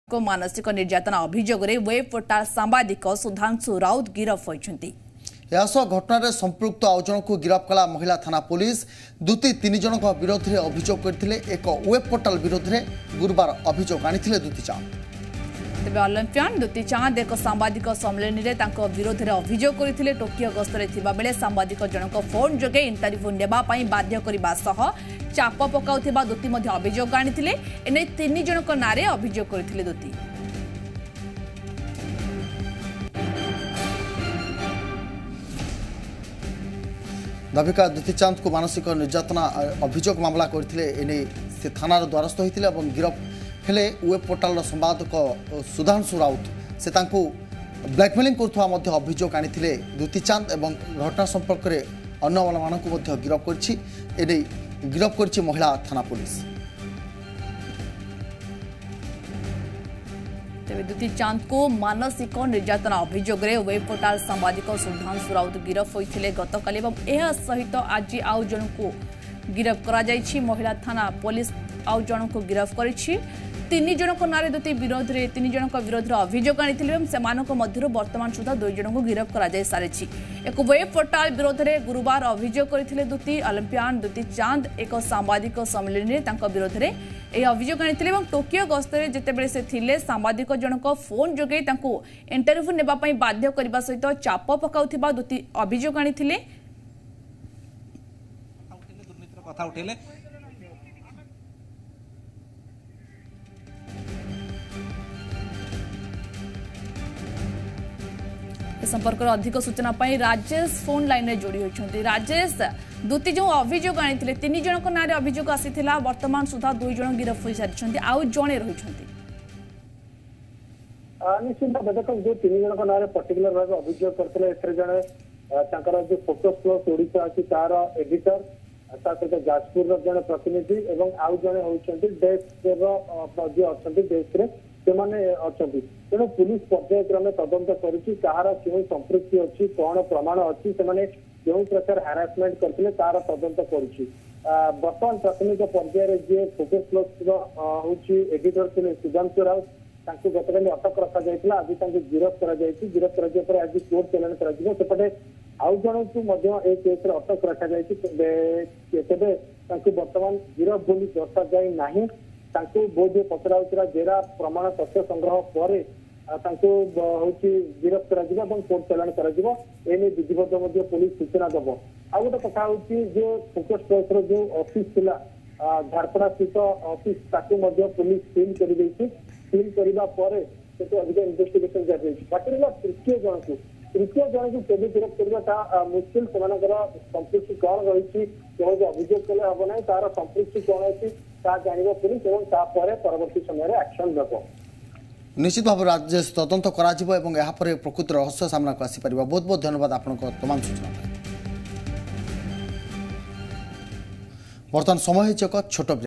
Olympian Dutee Chand's Defamation Case । ୱେବ ପୋର୍ଟାଲ Editor Sudhansu Rout ଗିରଫ । ଦୂତି ଚାନ୍ଦ
ଦୂତୀ ଚାନ୍ଦଙ୍କୁ ମାନସିକ ନିର୍ଯ୍ୟାତନା ଅଭିଯୋଗରେ ୱେବ୍ ପୋର୍ଟାଲ ସାମ୍ବାଦିକ ସୁଧାଂଶୁ ରାଉତ ଗିରଫ । ଏକ ୱେବ୍ ପୋର୍ଟାଲ ବିରୋଧରେ ଗୁରୁବାର ଅଭିଯୋଗ କରିଥିଲେ ଦୂତି ଚାନ୍ଦ । ଗତକାଲି ଅଲମ୍ପିଆନ୍ ଦୂତୀ ଚାନ୍ଦ ଏକ ସାମ୍ବାଦିକ ସମ୍ମିଳନୀରେ ତାଙ୍କ ବିରୋଧରେ ଅଭିଯୋଗ କରିଥିଲେ । ଟୋକିଓ ଗସ୍ତରେ ଥିବାବେଳେ ସାମ୍ବାଦିକ ଜଣକ ଫୋନ୍ ଯୋଗେ ଇଣ୍ଟରଭ୍ୟୁ ନେବା ପାଇଁ ବାଧ୍ୟ କରିବା ସହ ଚାପ ପକାଉ ଥିବା ଦୂତୀ ଅଭିଯୋଗ କରିଥିଲେ । ଏନେଇ ୩ ଜଣଙ୍କ ନାମରେ ଅଭିଯୋଗ କରିଥିଲେ ଦୂତୀ । ସେପଟେ ୱେବ ପୋର୍ଟାଲ ସାମ୍ବାଦିକ ସୁଧାଂଶୁଙ୍କ ସହ ଆଉ ଜଣେ ମଧ୍ୟ ଗିରଫ ହୋଇଥିବା ସୂଚନା ରହିଛି ।

About Channel:-